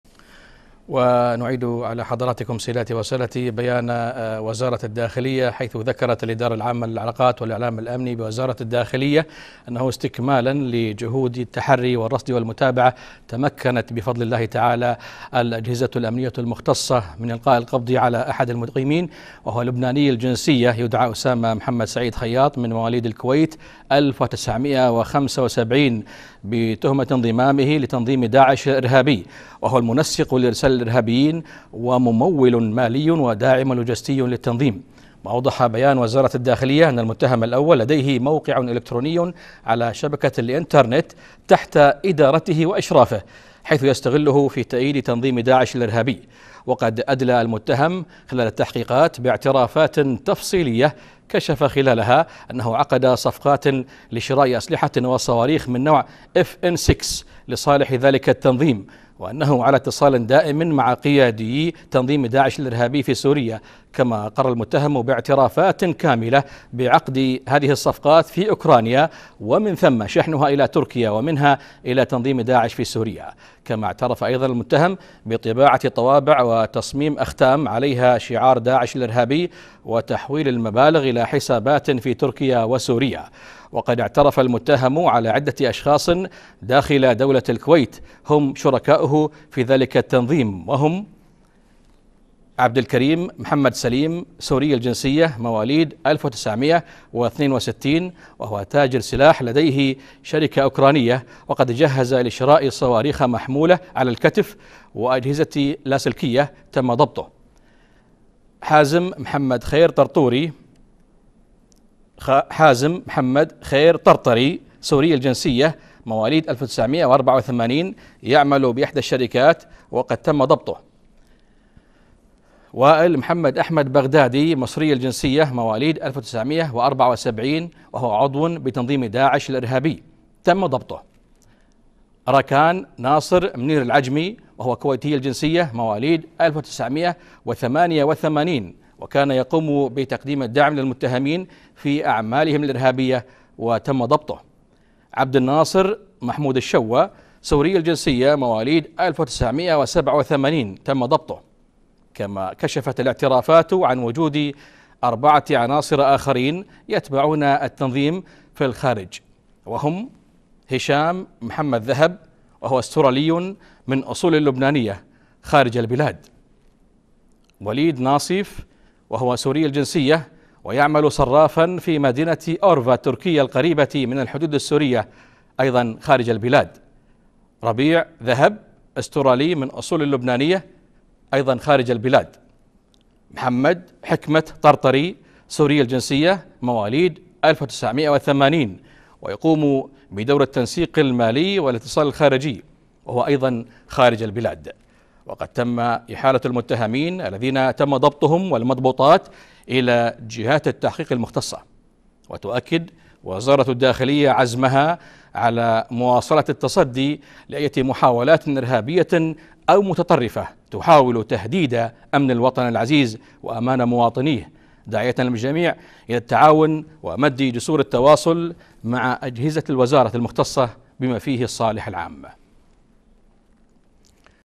بيان وزارة الداخلية حول القبض على خلية إرهابية لـداعش من 6 أشخاص بينهم كويتي
تمكنت الأجهزة الأمنية المختصة من إلقاء القبض على مقيم لبناني يدعى أسامة محمد سعيد خياط (من مواليد الكويت 1975)، بتهمة انضمامه لتنظيم داعش الإرهابي، وهو المنسق لإرسال الإرهابيين وممول مالي وداعم لوجستي للتنظيم.